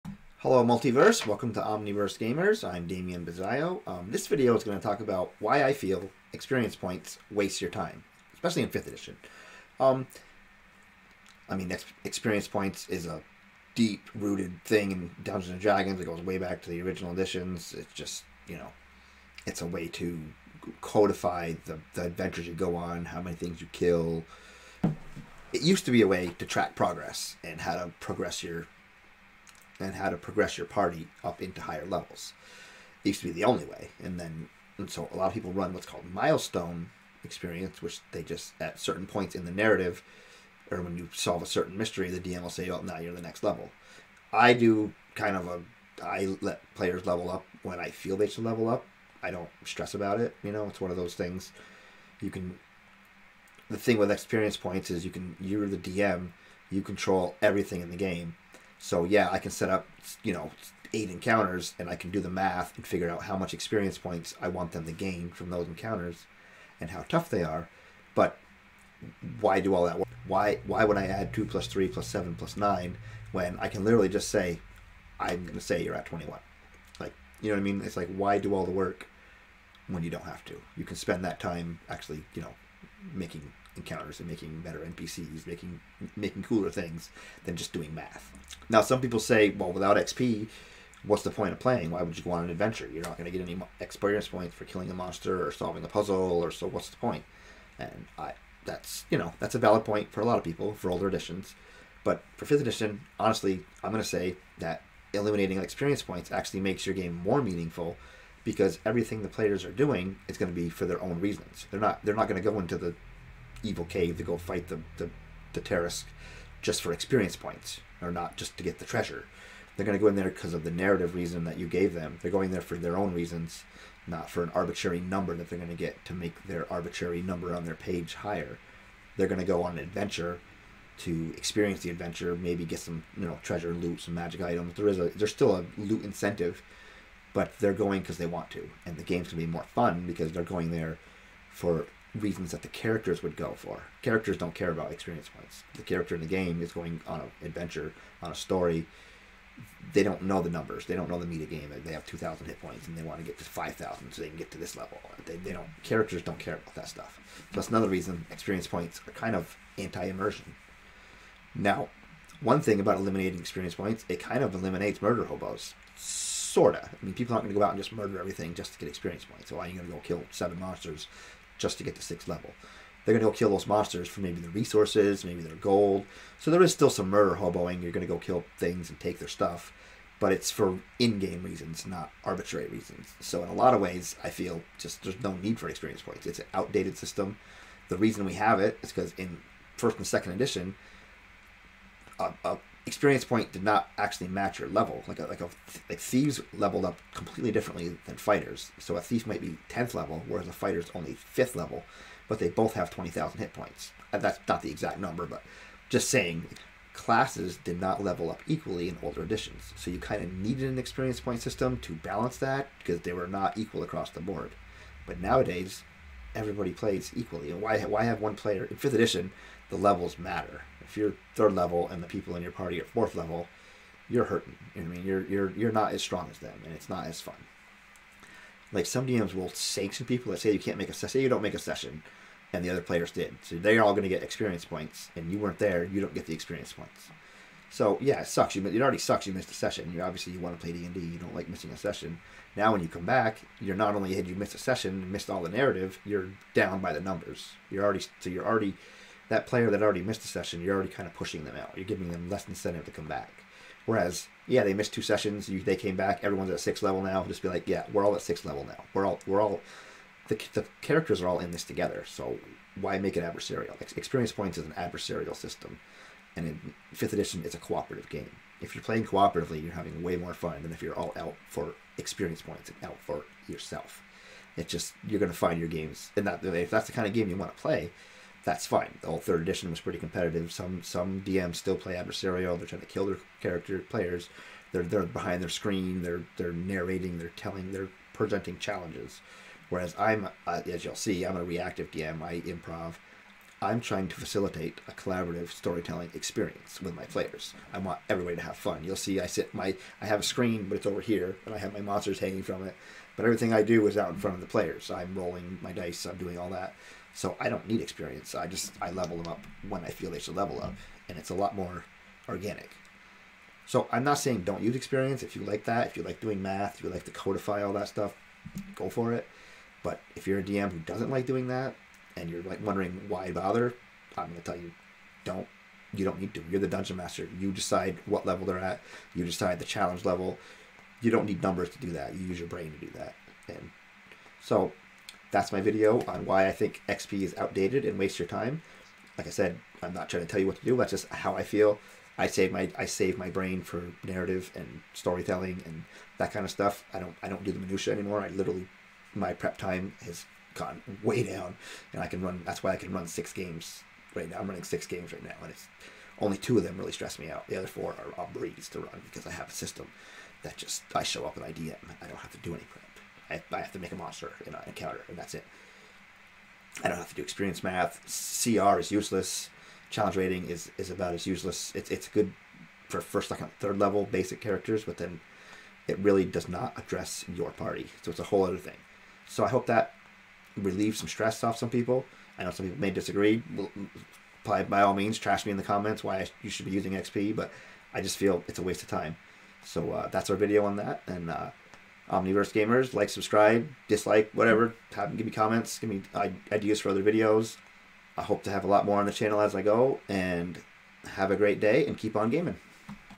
XP Wastes Your Time - DM tips - 5e
This is my opinion on why XP is an outdated system and a waste of a DM's time.